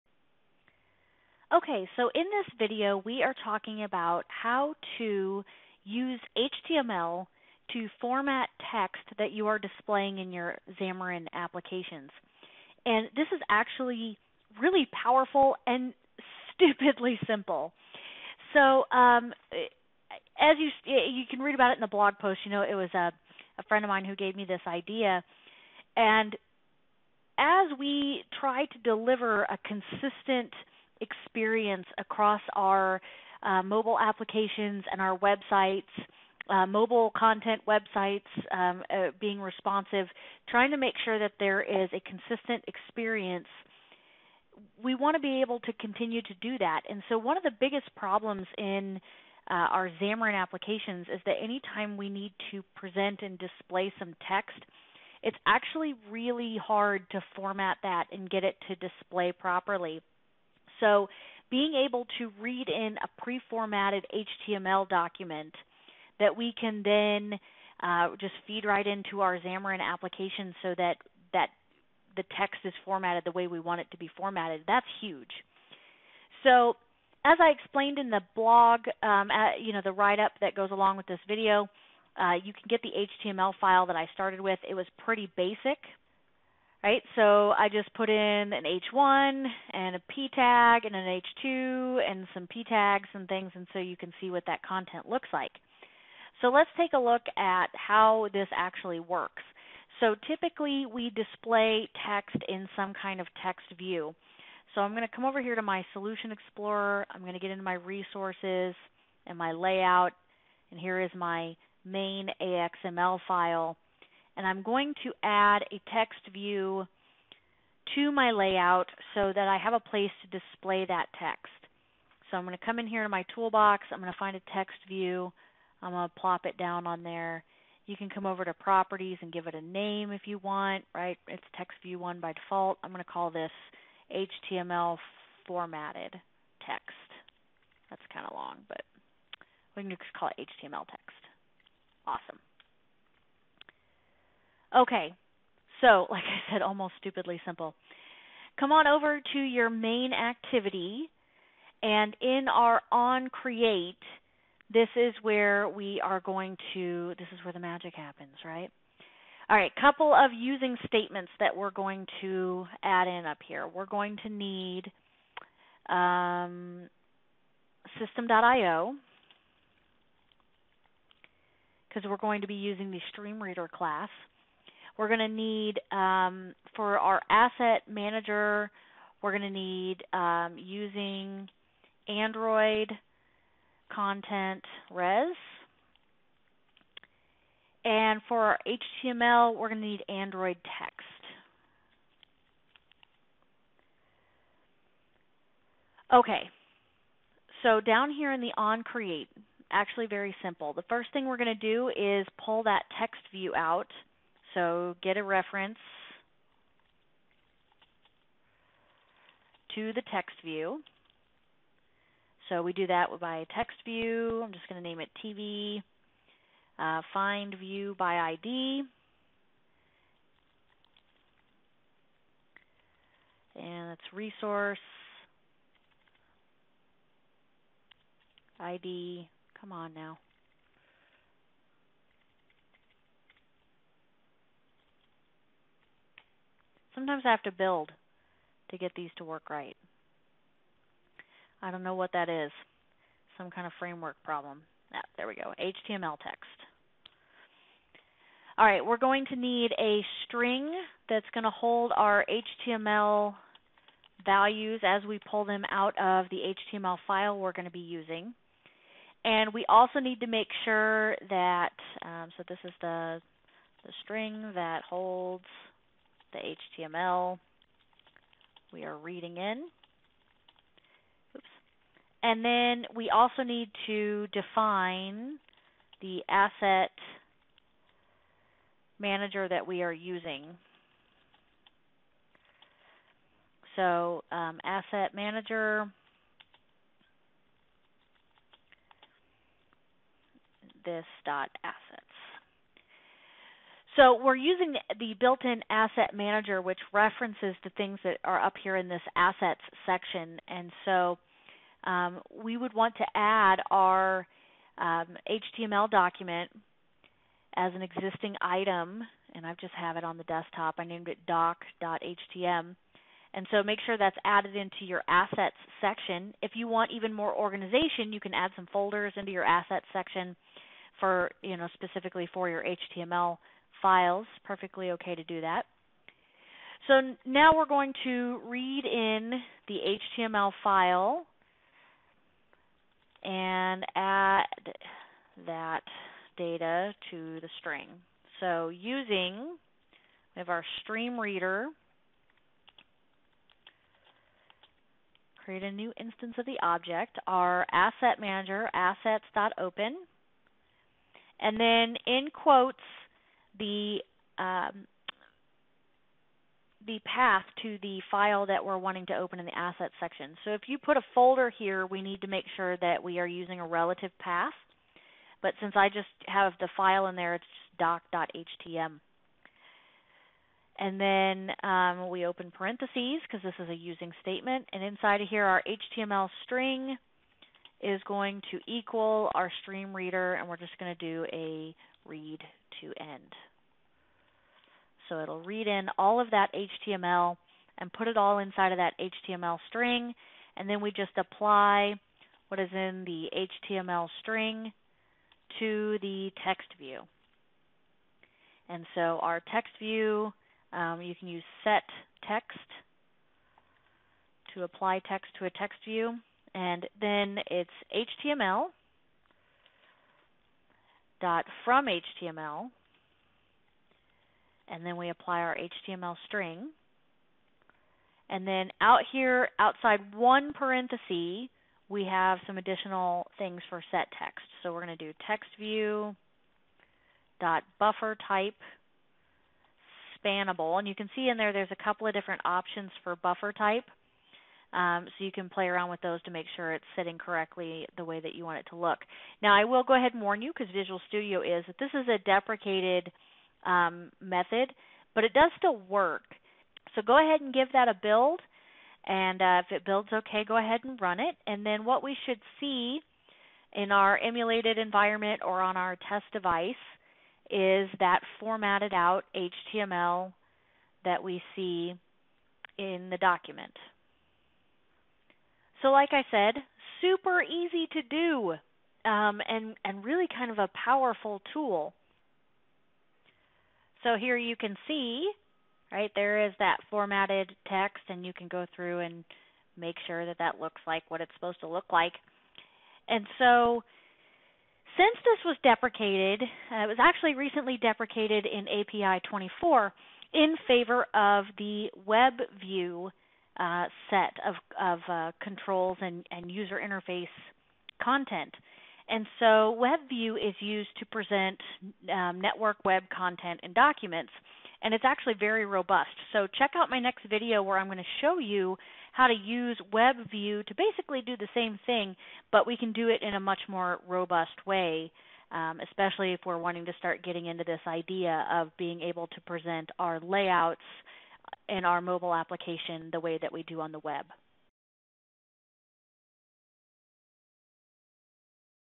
Xamarin Android - Reading HTML into a TextView
This video shows you to how take pre-formatted text as HTML and read it into a TextView in your Xamarin Android applications so you can preserve the formatting.